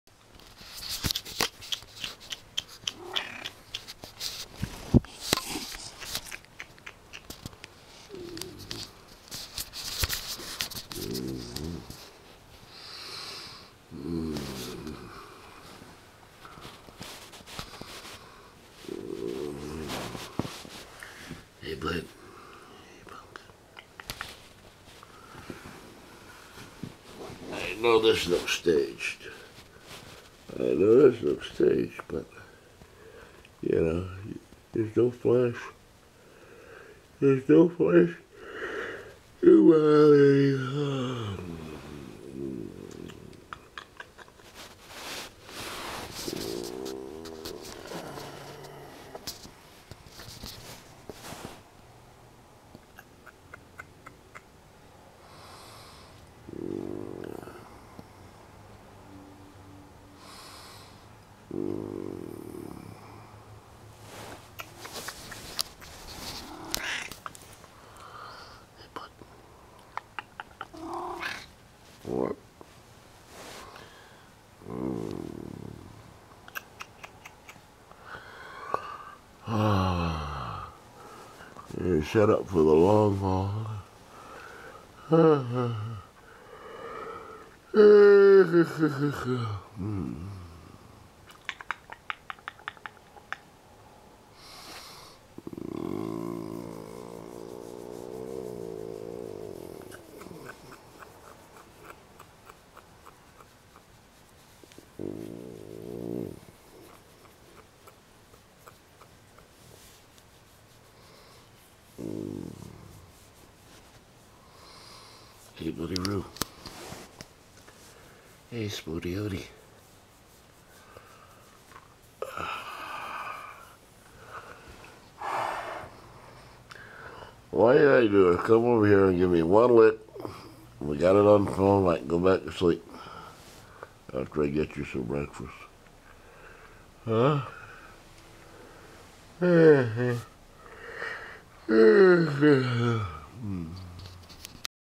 Licks From BB King
Early Sunday morning, BB alerts me to his empty kibbles bowl.  This elusive ritual finally captured on film.  ;)